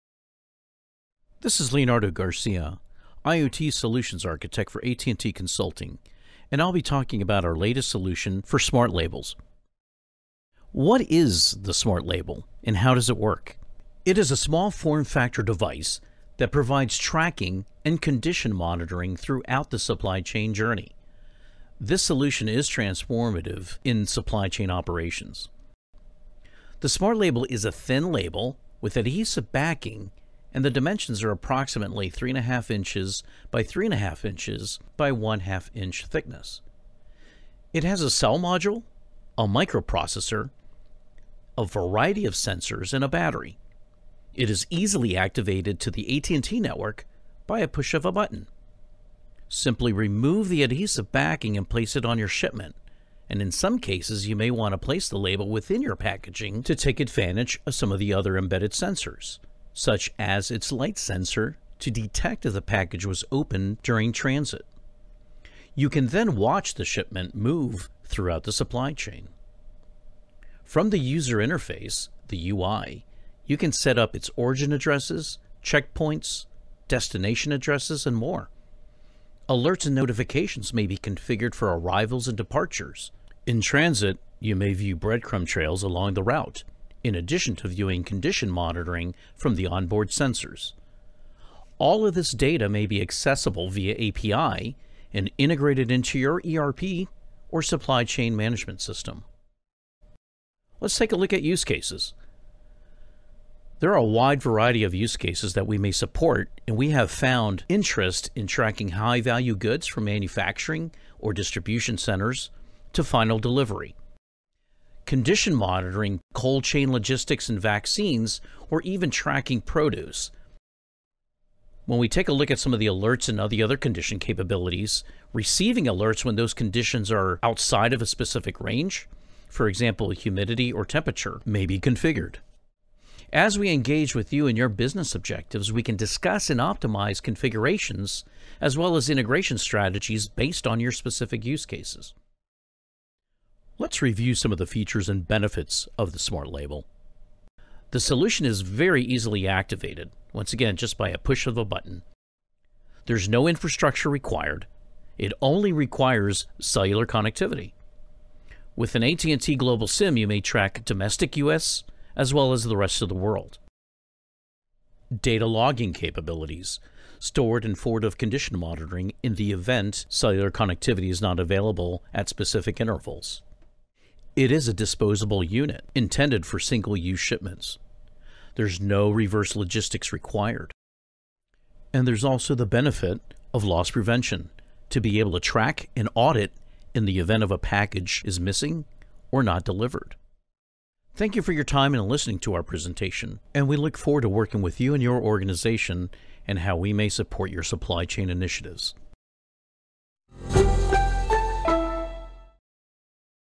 AT&T Smart Label solution overview
Leonardo Garcia provides an in-depth look at the AT&T Smart Label solution and how it can help you track, monitor, and manage your supply chain.